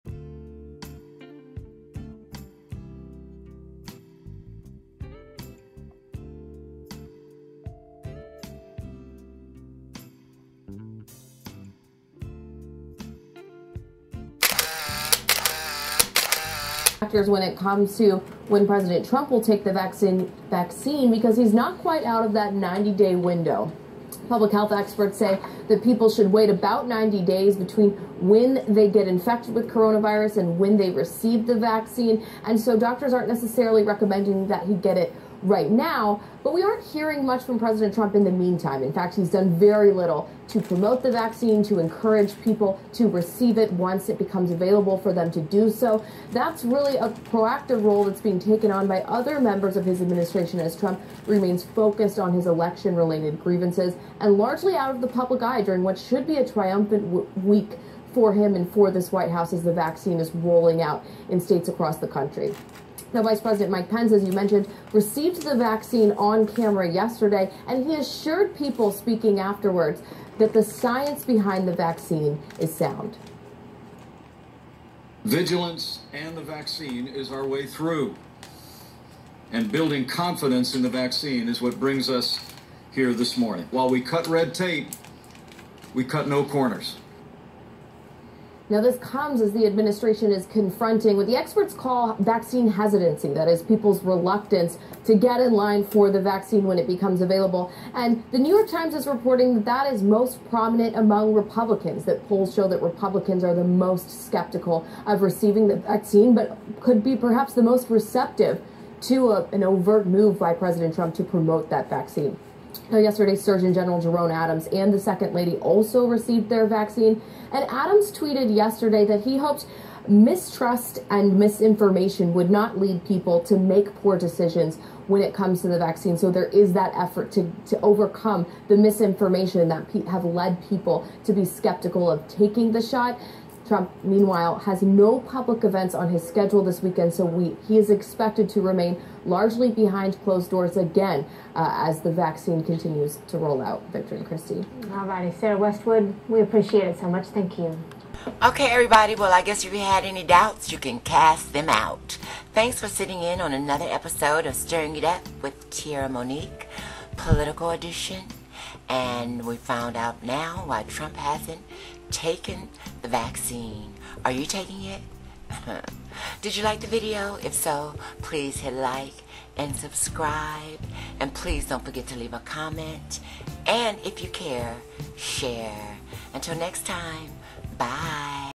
Reason why President Trump hasn't taken the Vaccine.
We have wondered why President Trump hasn't signed up for his round of Covid shots is because you have to wait 90 after having the disease to be vaccinated. What do you think?